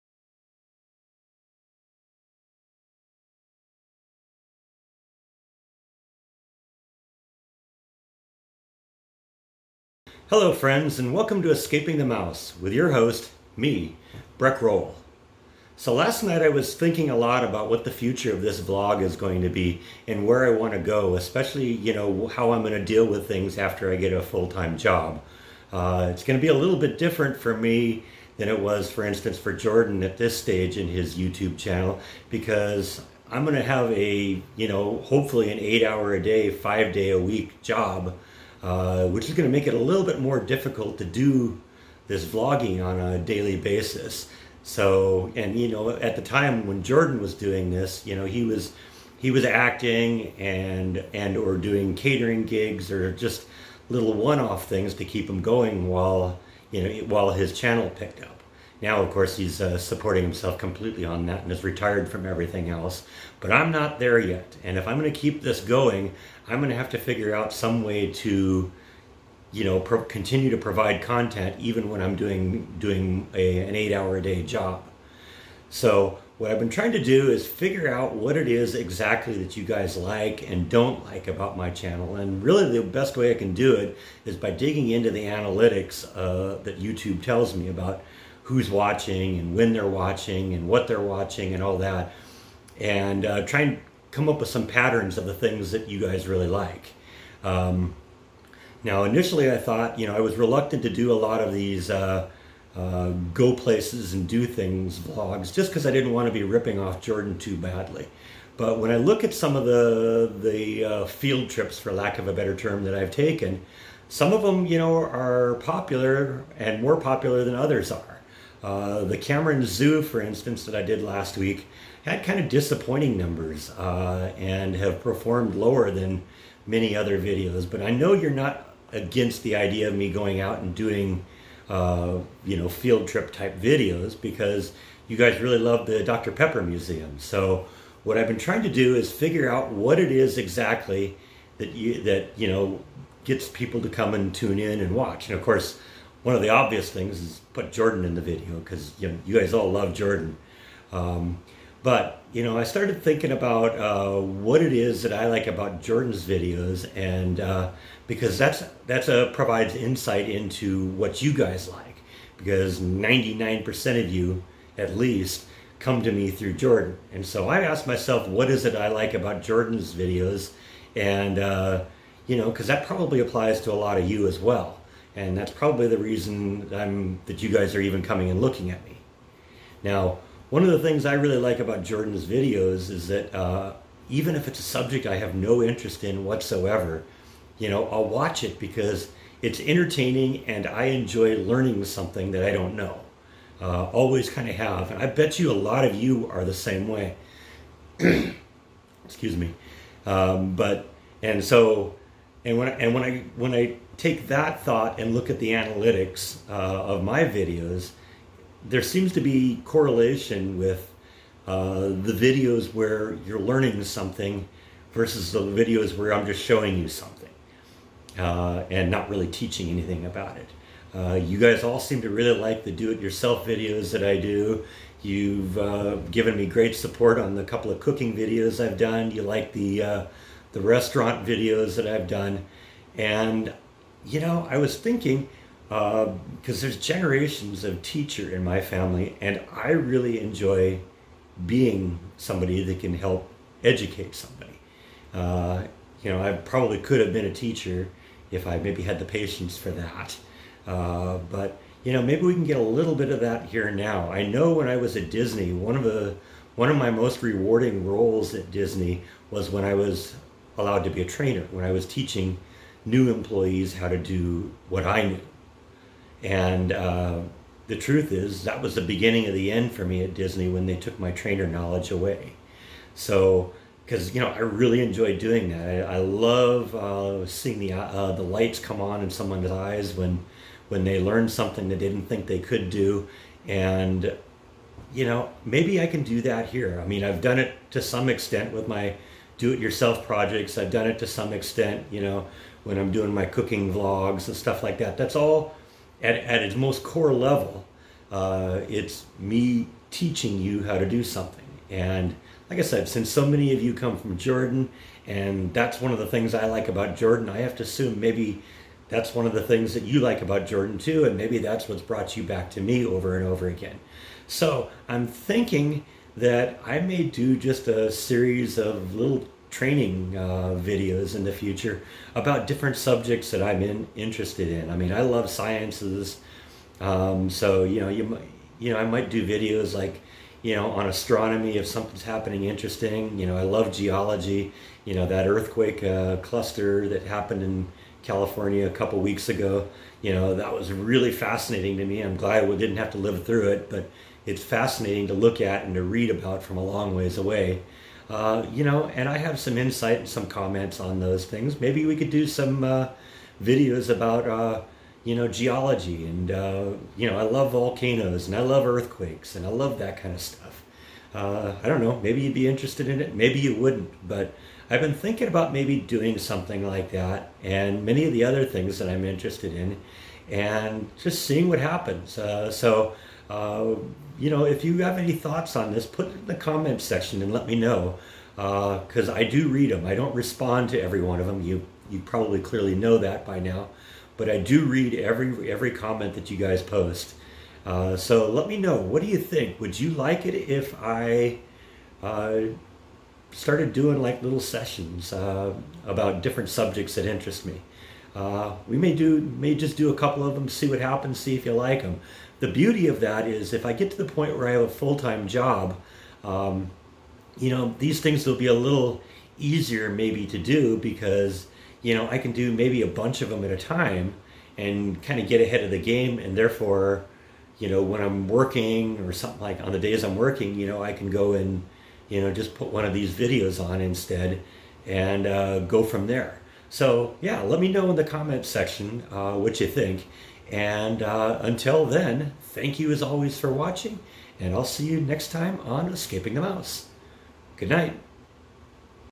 Where Do We Go From Here? - Escaping The Mouse - ETM-86
Today I ponder what the future looks like for this vlog and how will I continue once I have a full time job.